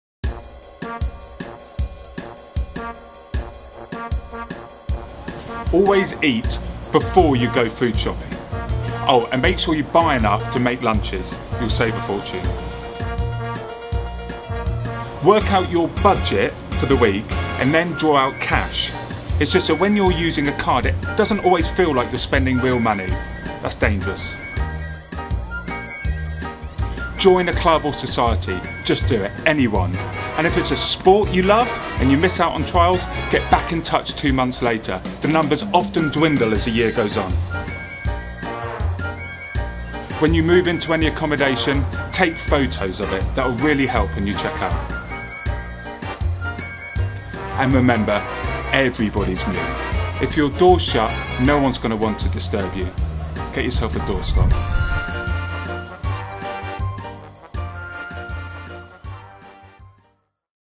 5 Top Tips For Uni Life
Hey Students! Here's 5 top tips for uni life from our Student Pastor, Rob!